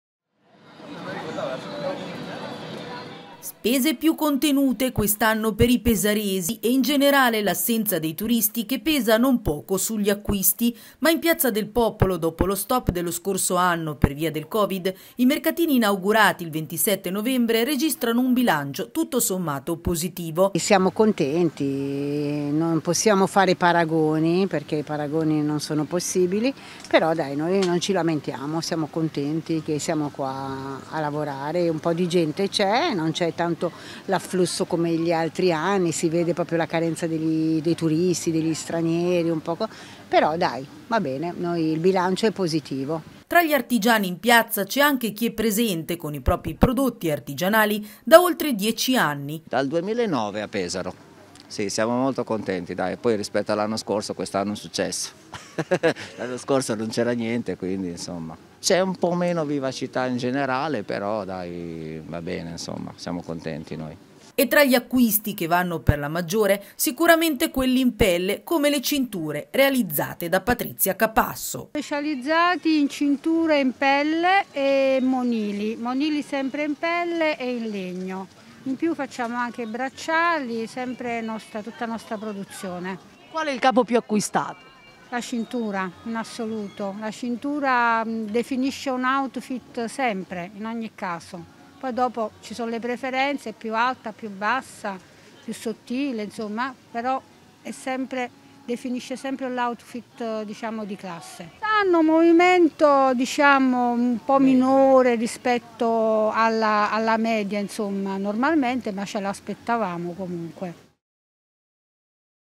Pesaro, bilancio positivo per i mercatini
Dopo la pausa dello scorso anno per via della pandemia, i mercatini natalizi, dislocati tra piazza del Popolo e vie limitrofe, in centro storico a Pesaro, quest'anno hanno registrato un bilancio positivo.